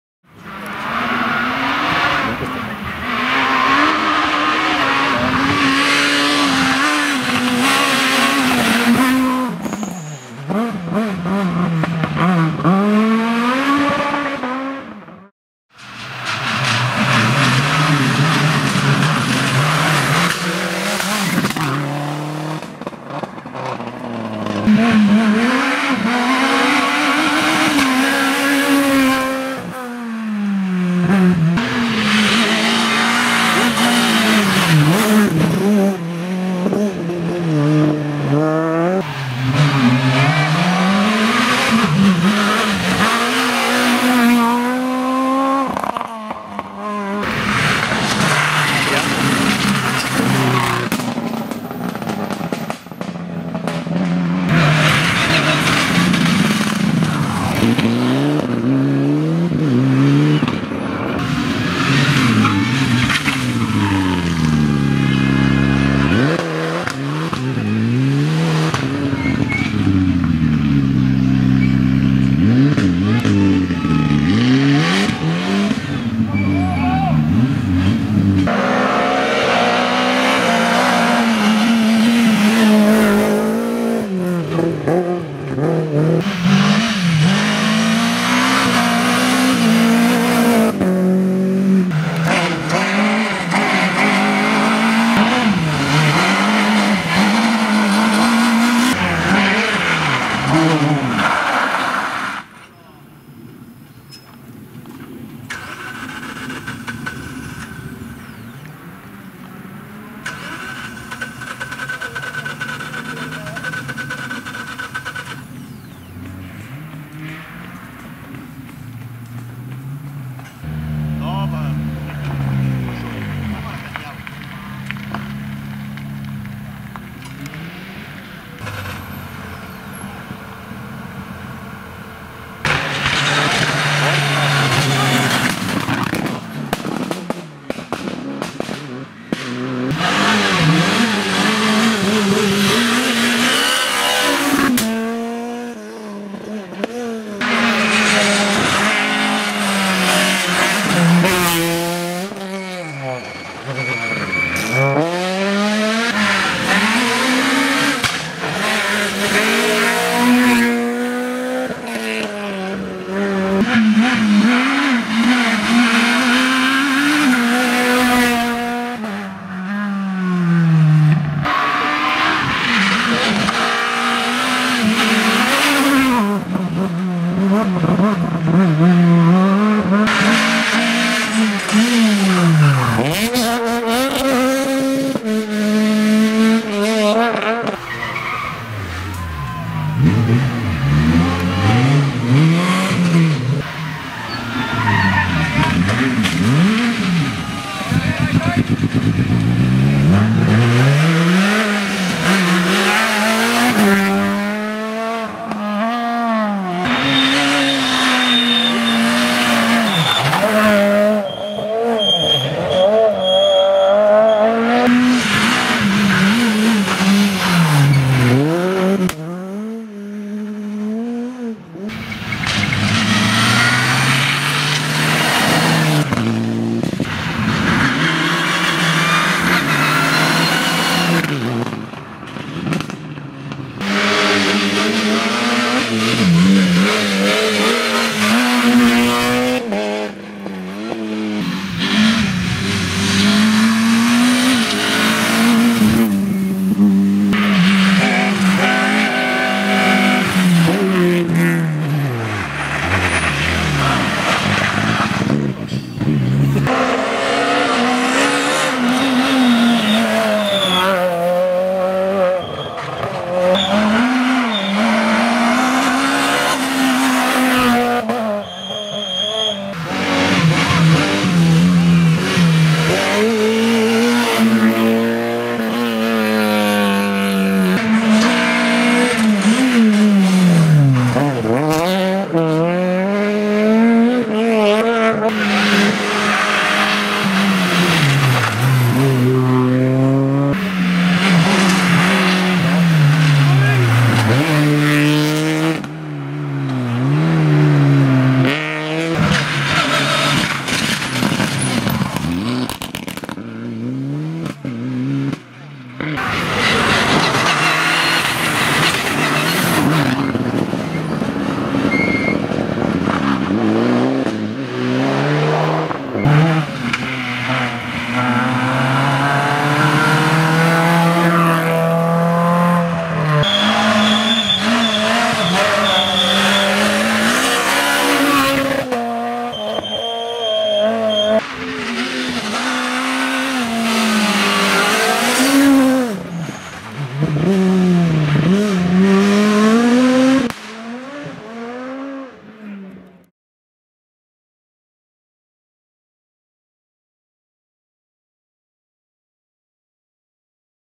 Rally di Castiglione Torinese 2018 | Action, Crash & Mistakes [HD]
• 2° Rally Day di Castiglione Torinese 2018 •

Results:
1° Zippo-Ceschino | Renault Clio S1600
2° Pinzano-Cerutti | Renault Clio R3C
3° Fioroni-Rocchi | Renault Clio R3C